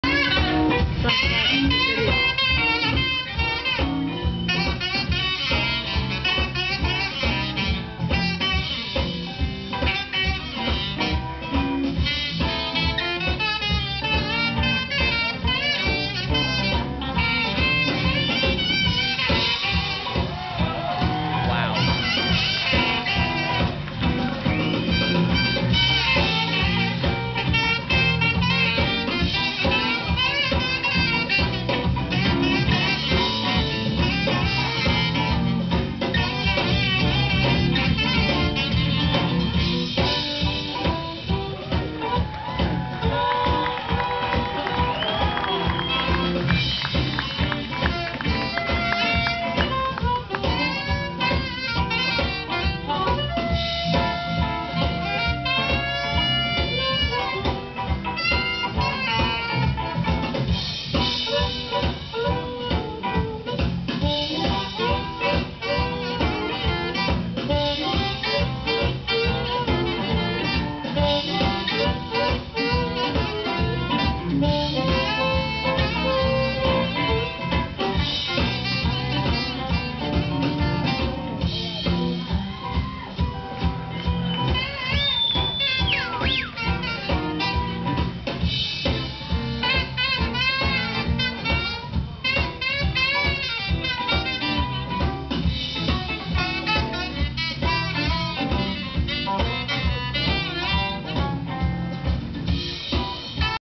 Grace Kelly w/ James Cotton @ B.B. King Blues Club 1-11-07
Sort clip of teen saxophonist Grace Kelly joining bluesman James Cotton @ B.B. King Blues Club, NYC on 1-11-07